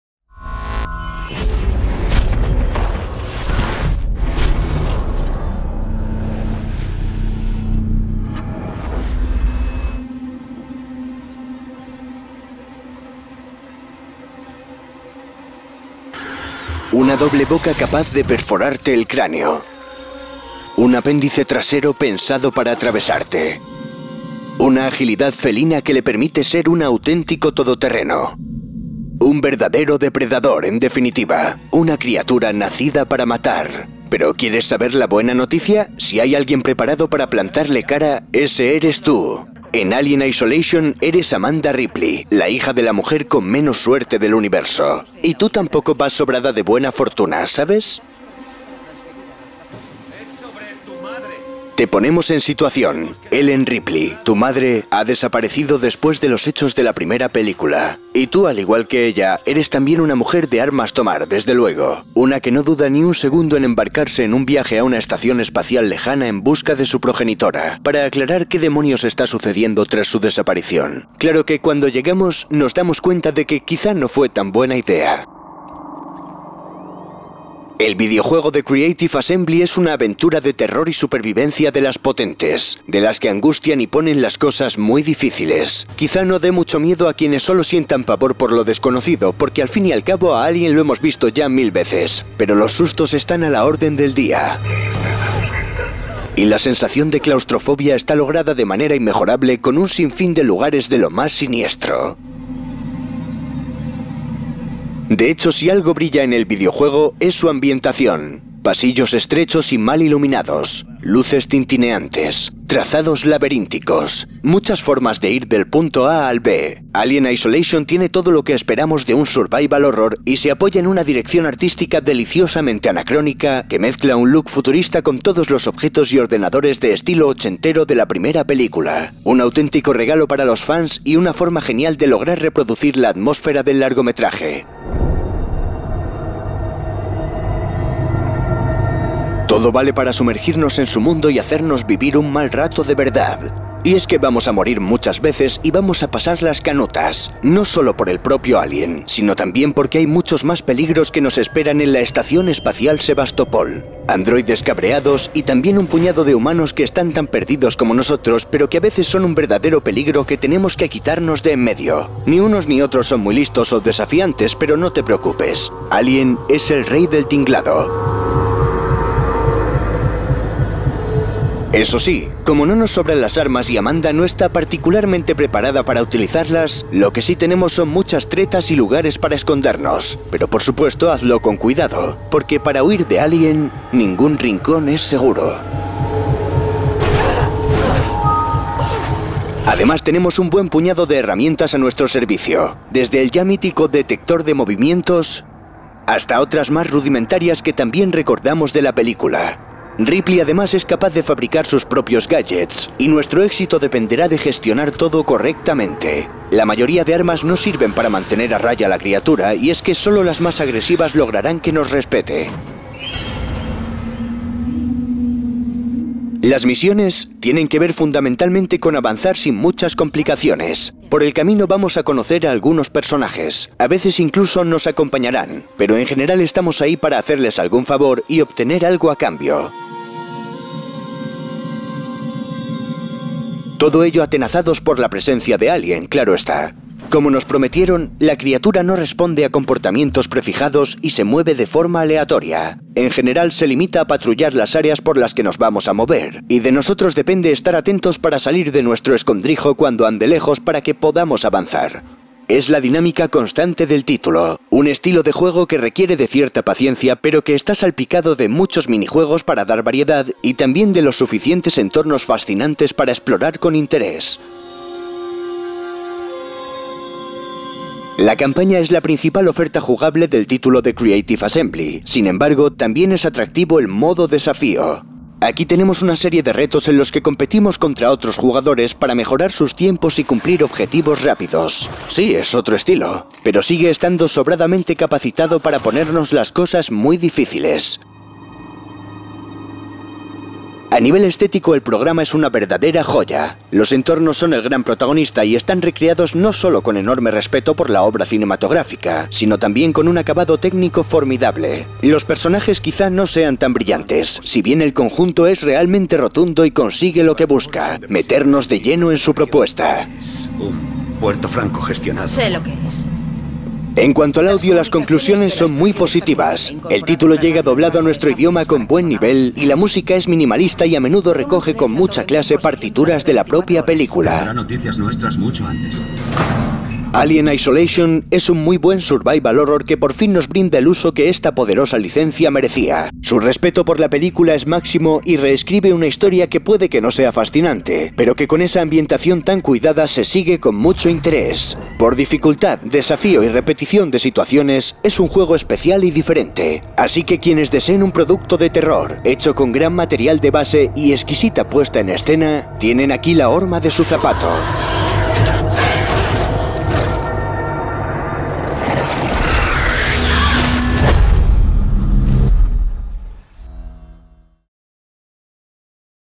Análisis de Alien Isolation - "Suspense máximo y angustia infinita"
¿Un juego de Alien que merece la pena? ¡Desde luego! Echa un vistazo a nuestro análisis en vídeo para Alien: Isolation, la primera y apasionante incursión de los creadores de Total War en el survival horror.

Análisis de Alien Isolation (en español)